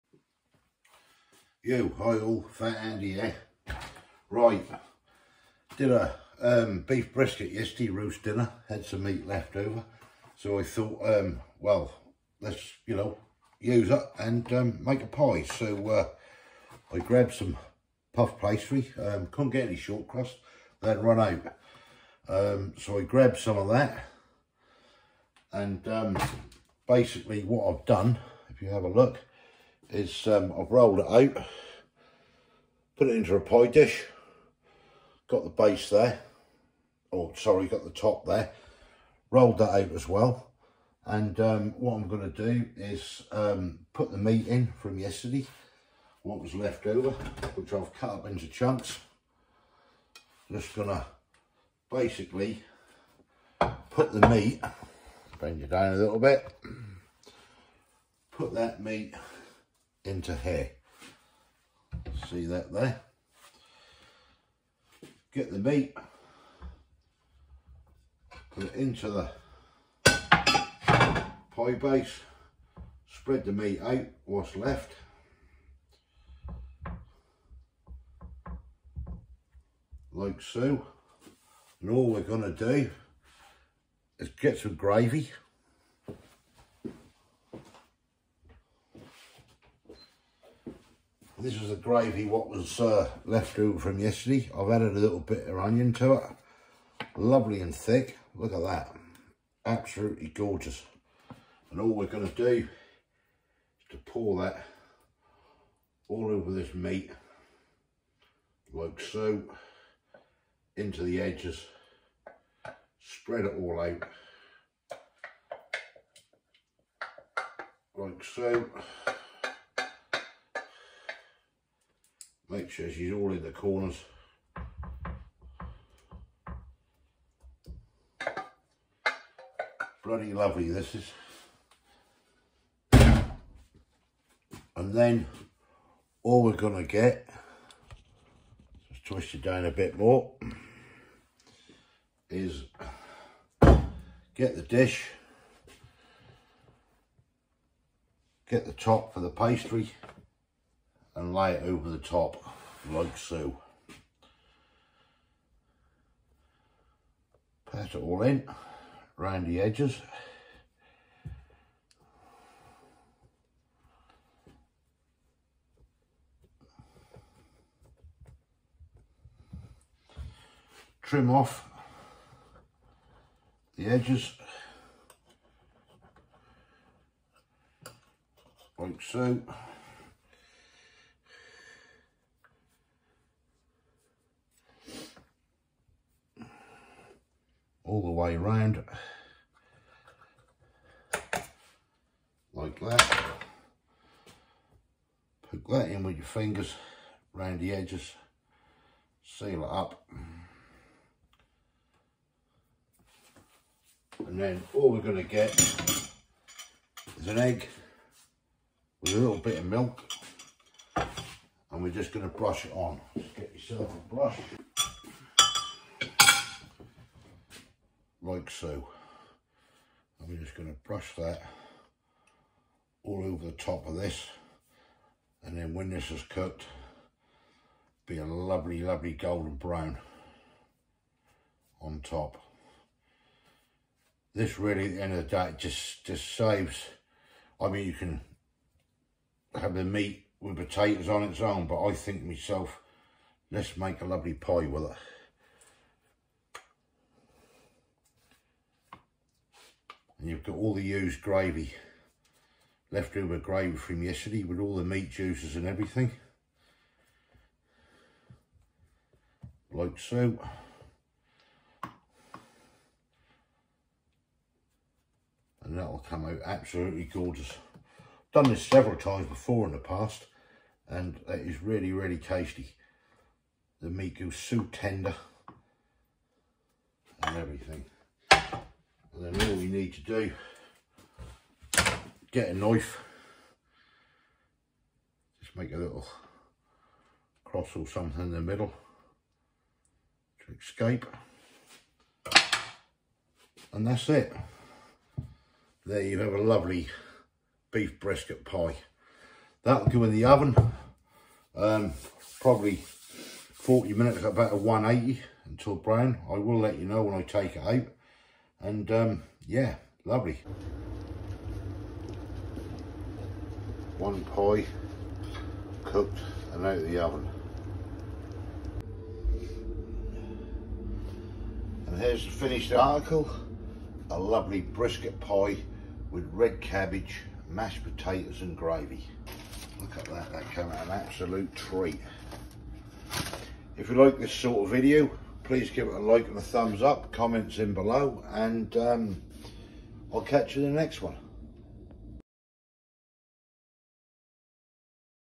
Homemade Beef Brisket Pie
Beef brisket pie made from left over Sunday roast, delicious